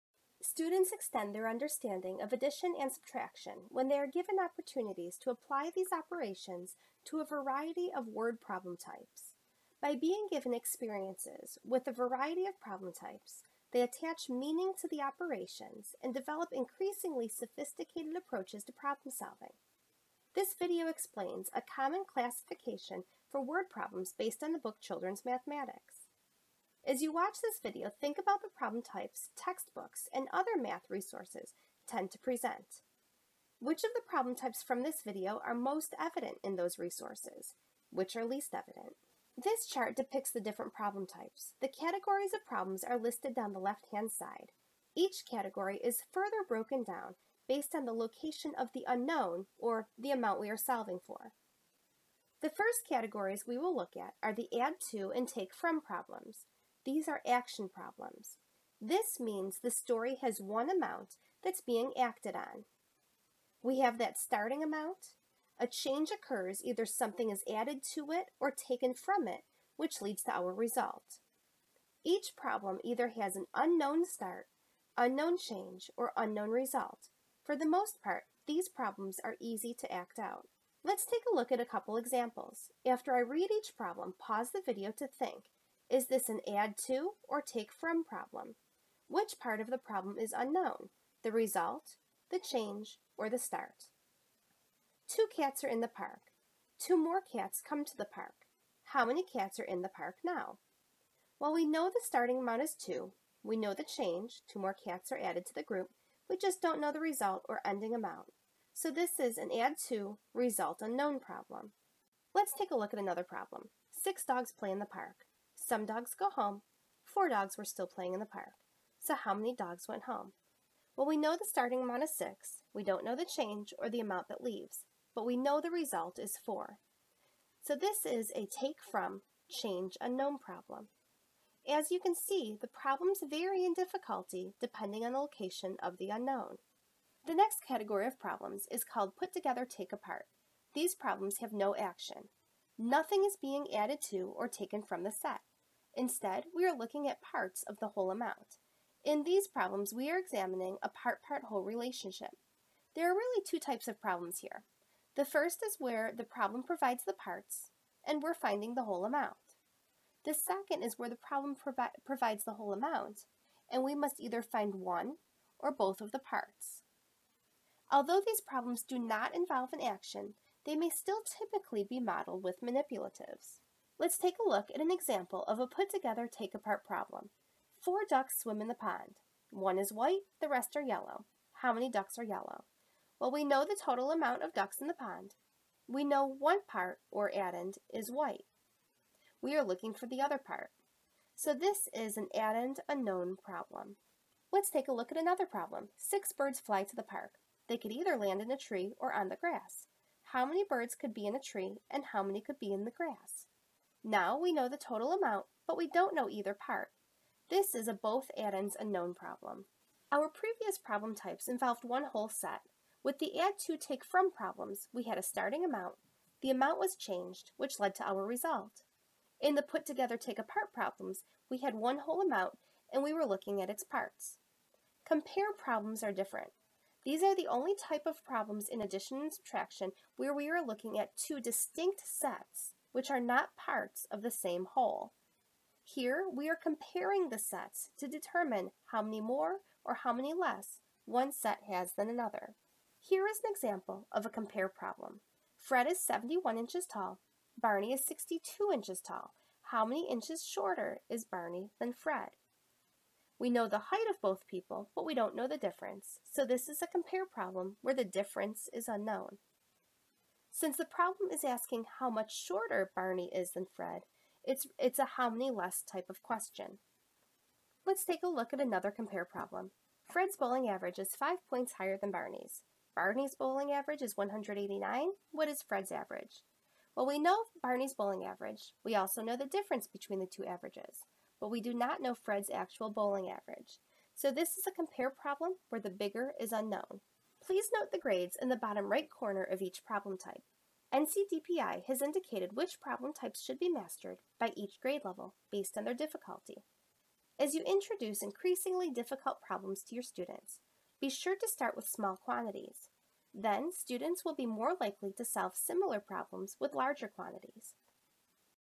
Addition and Subtraction Problem Types
This video looks at a common classification for addition and subtraction problem types based on the book Children's Mathematics.